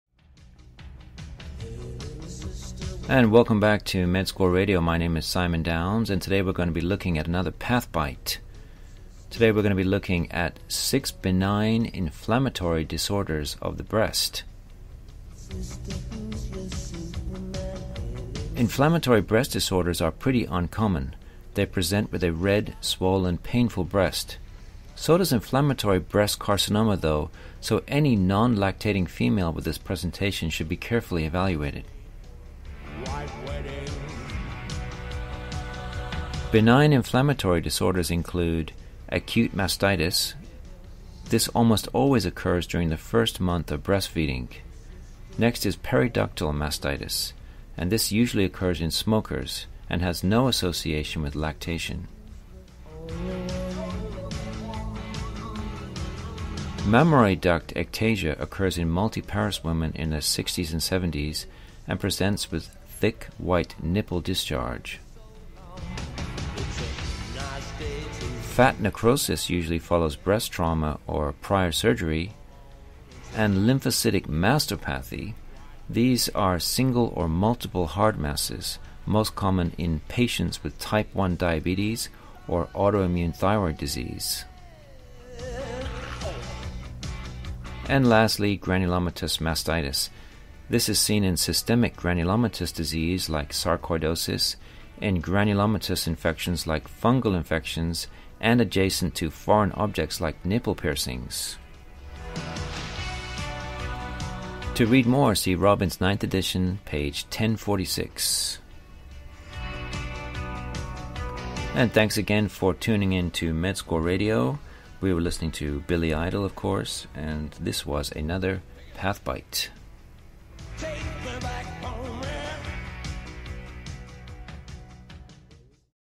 Path Bites: Six benign inflammatory disorders of the breast
Inflammatory breast disorders are pretty uncommon. They present with a red, swollen, painful breast. So does inflammatory breast carcinoma, though - so any non-lactating female with this presentation should be carefully evaluated.

Benign inflammatory disorders include:

Acute mastitis (almost always occurs during the first month of breastfeeding)

Periductal mastitis (usually occurs in smokers; no association with lactation)

Mammary duct ectasia (occurs in multiparous women in 60s-70s; presents with thick white nipple discharge)

Fat necrosis (usually follows breast trauma or prior surgery)

Lymphocytic mastopathy (single or multiple hard masses; most common in patients with type I diabetes or autoimmune thyroid disease)

Granulomatous mastitis (seen in systemic granulomatous disease like sarcoidosis; in granulomatous infections like fungal infections; and adjacent to foreign objects like nipple piercings)